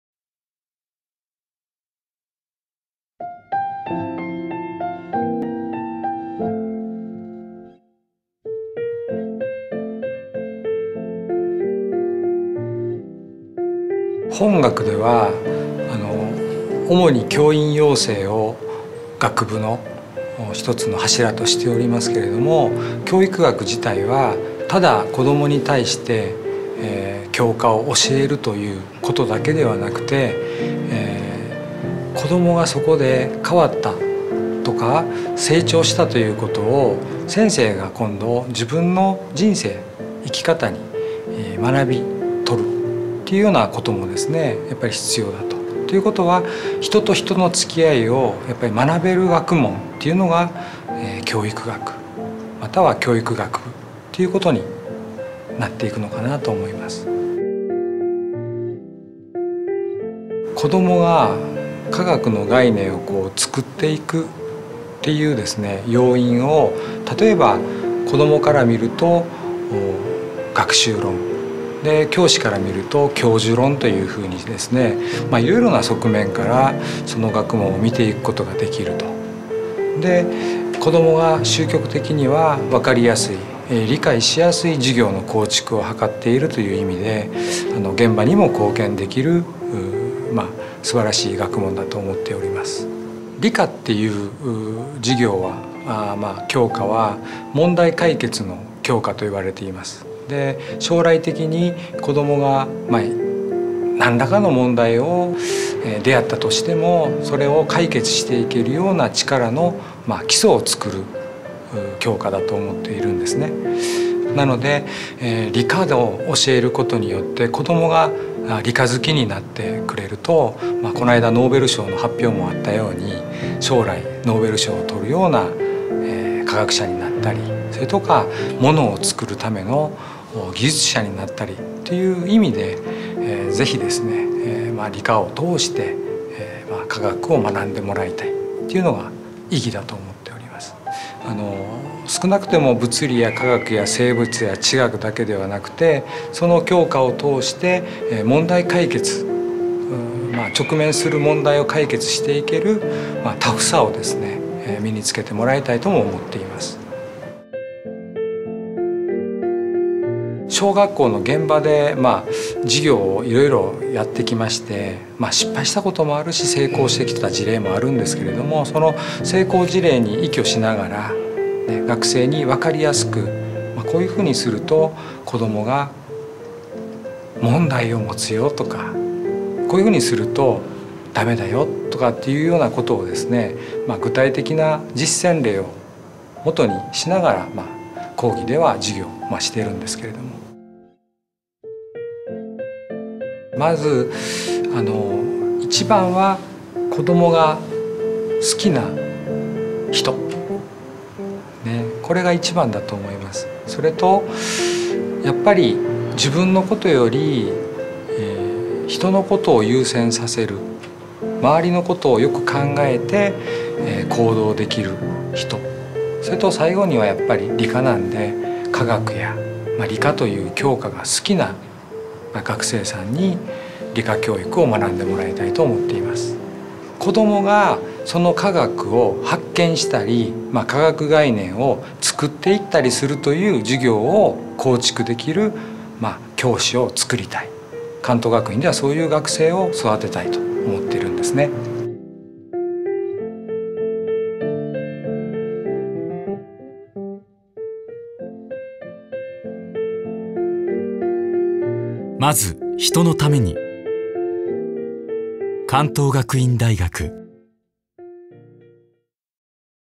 関東学院チャンネル [大学篇 Episode52]
黒田 篤志
教育学部 こども発達学科 准教授

大阪府出身。専門は理科教育学。
20年以上にわたって横浜市内の公立小学校に勤務し、
子どもたちの成長を見守ってきた。現職着任後は、自身の経験も生かしながら
次の世代の「先生」を育てるために尽力する。

※教育学部は、2015年4月スタート。
※プロフィール等は公開当時のものです。